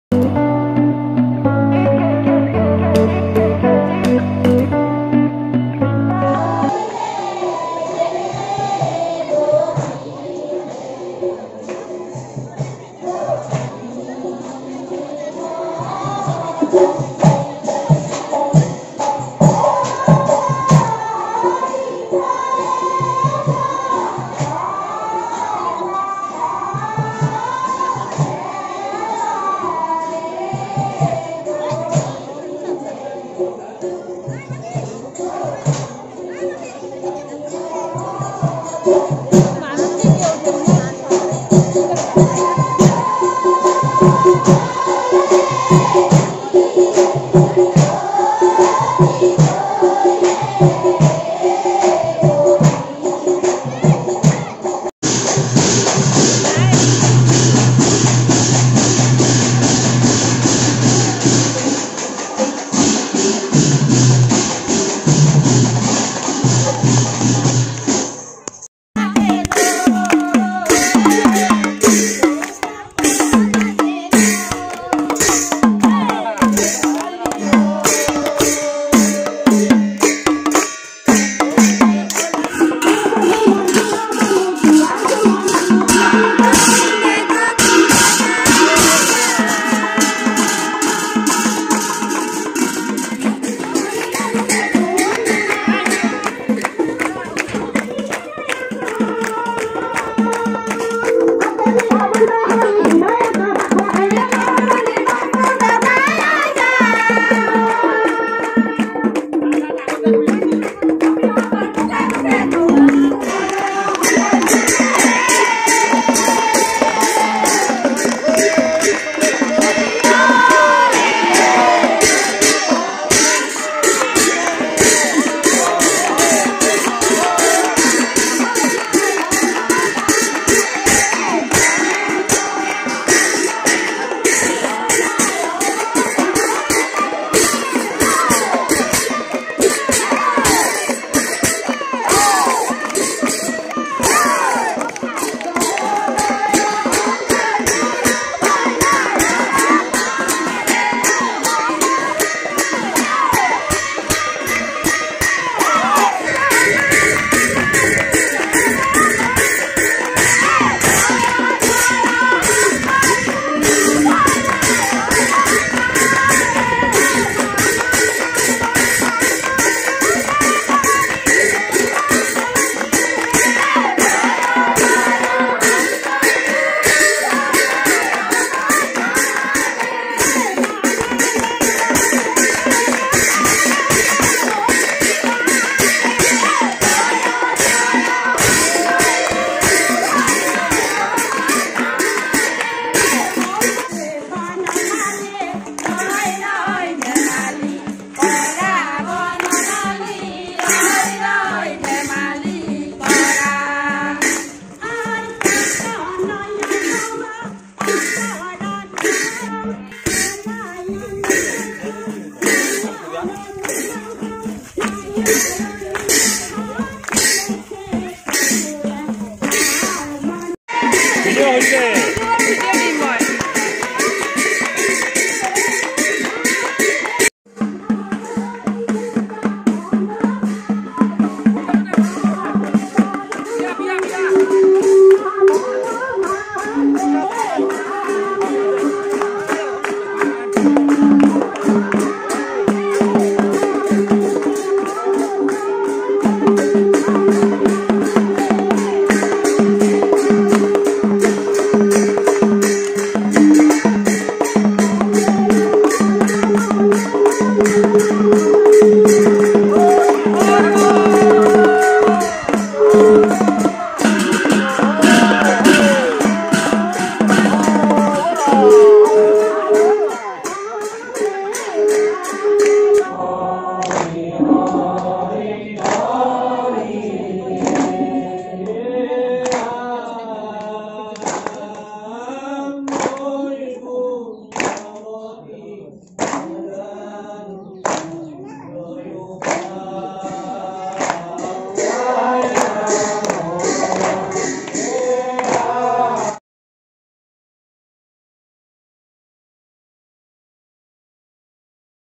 সাৰ্বজনীন পালনাম/ বিজয়গাঁও বৰছাৰা/ বাসুগাঁও/ Baruah Sisters kitchen and vlog🙏🙏
Dear viewers, welcome to Baruah Sisters kitchen and vlog. Today I am here with another vlog of Palnaam of Bijoygaon Barsara, Basugaon which is the home town of my dearest friend Smt Deepika Ray. My heartiest thanks to her giving her time to my channel.
So lets view the video and enjoy the rituals of Assam.

OUR PREVIOUS VLOG's